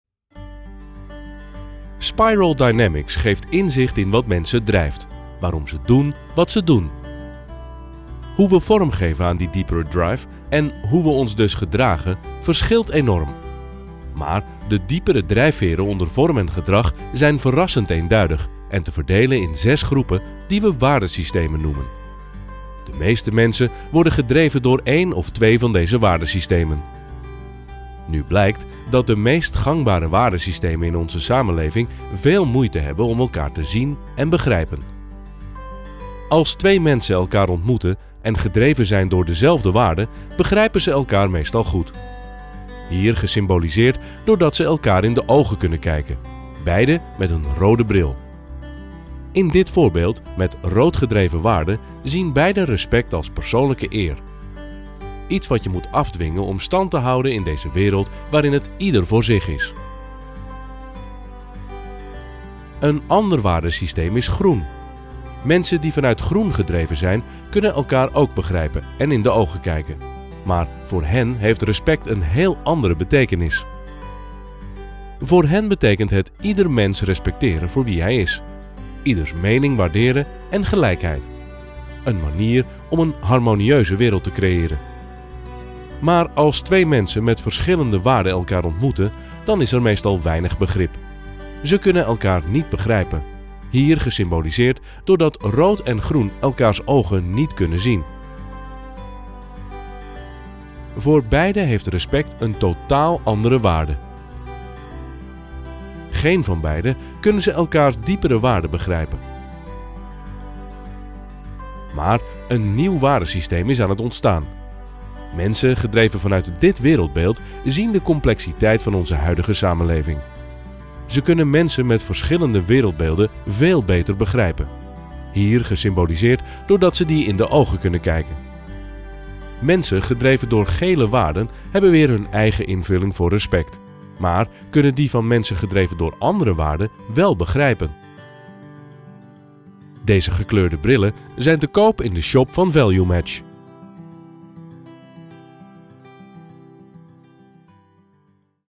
Spiral Dynamics kleurenbrillen
Spiral Dynamics is een model dat menselijke en maatschappelijke ontwikkeling beschrijft. ValueMatch levert assessment op basis van Spiral Dynamics voor persoonlijke coaching, teamwerk, organisatieontwikkeling en personeelswerk.